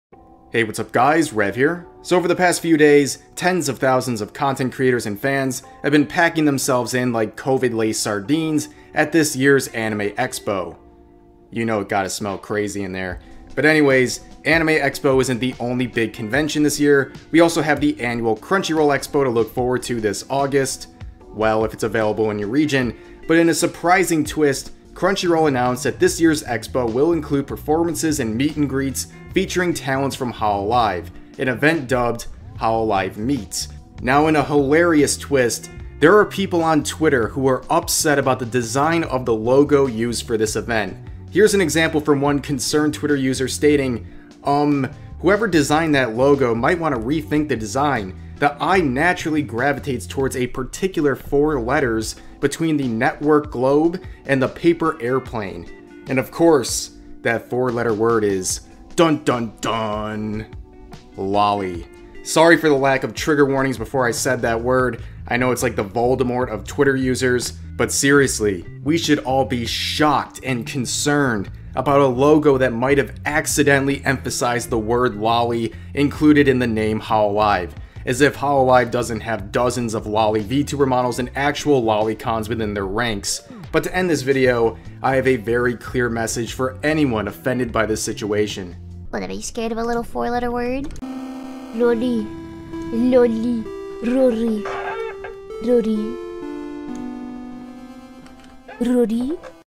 This Hololive x Crunchyroll "Controversy" Is Hilarious...
What are they scared of a little 4 letter word???